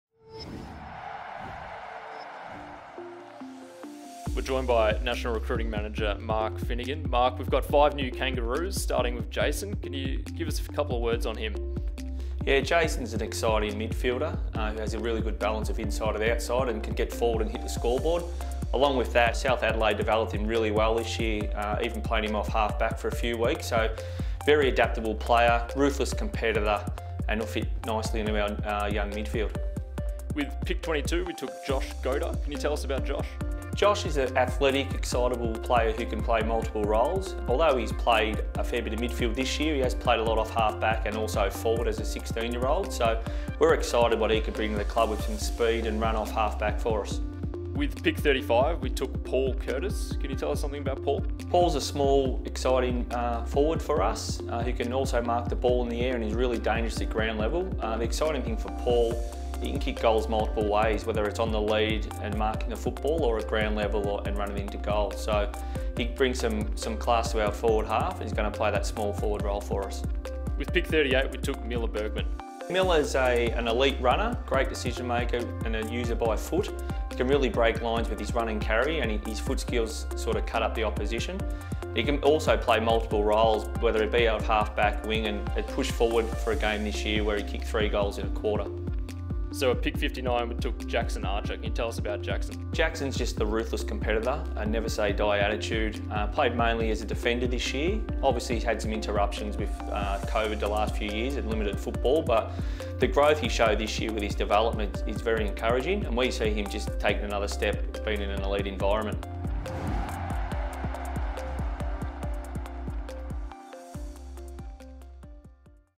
Mark Finnigan on North's draftees (November 26, 2021)
North Media sat down with National Recruiting Manager Mark Finnigan to discuss the five new faces joining the Kangaroos following the 2021 NAB AFL Draft.